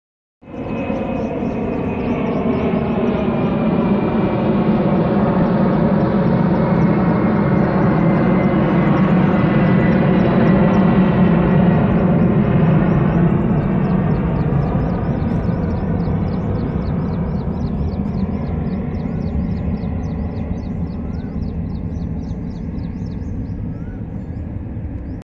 Boeing 747 take off from T3 Delhi | Peacocks at water body
Video from Anurag Bahl